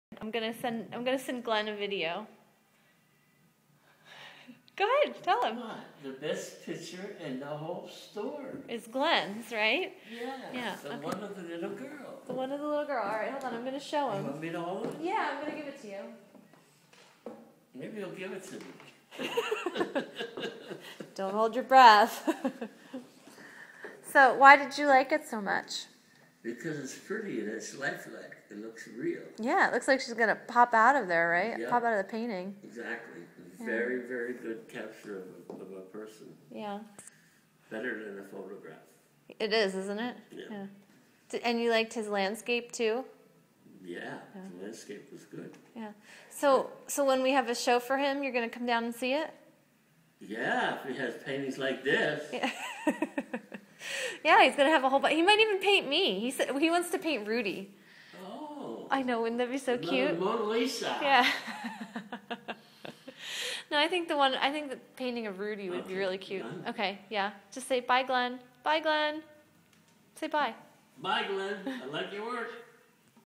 My dad on the art of Glenn Harrington
My Dad came to visit the gallery and loved the portraits by Glenn.